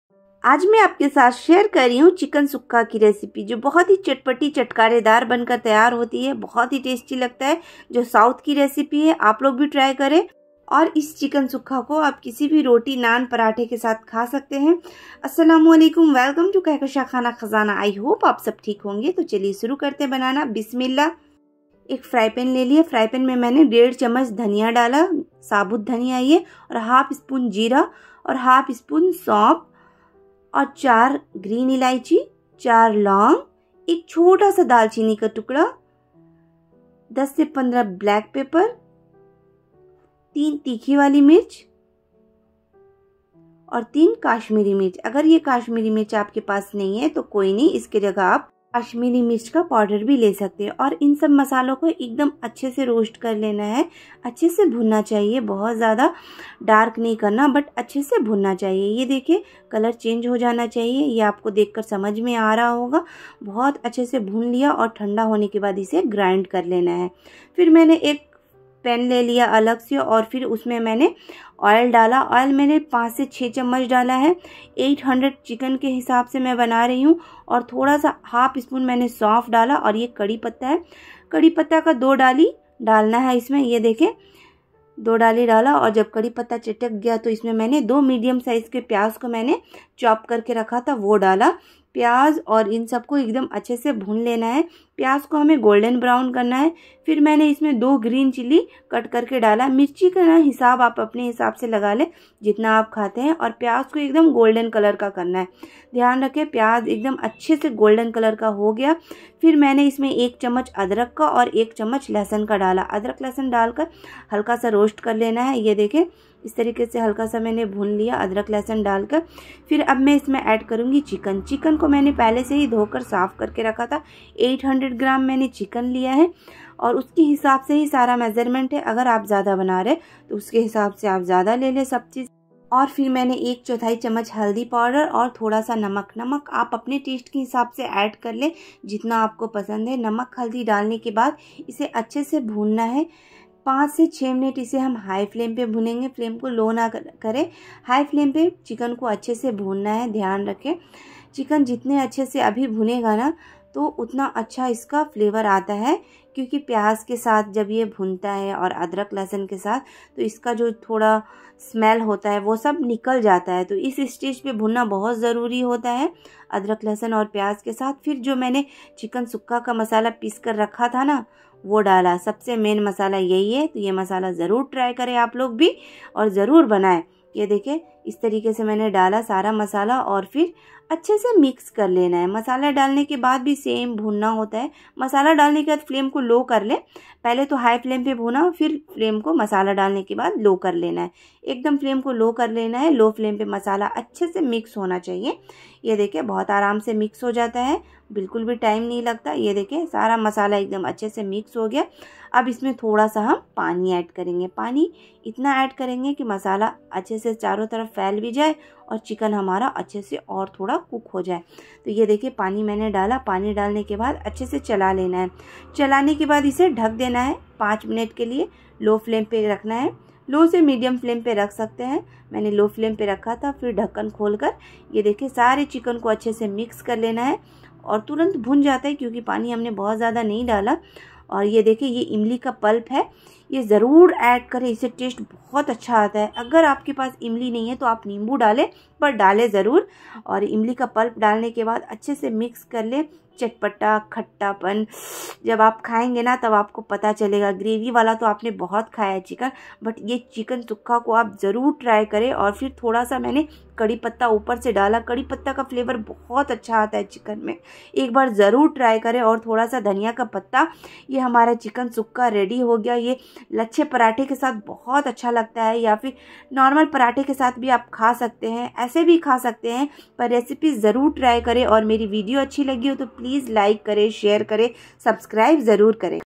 Chicken sukka recipe |Chicken masala roast recipe |South special chicken by Kk khana khazana
Chicken sukka recipe |Chicken masala roast recipe |South special chicken by Kk khana khazana South ki special chicken sukka recipe aap log jarur try karen different type of recipe chicken roast yummy 😋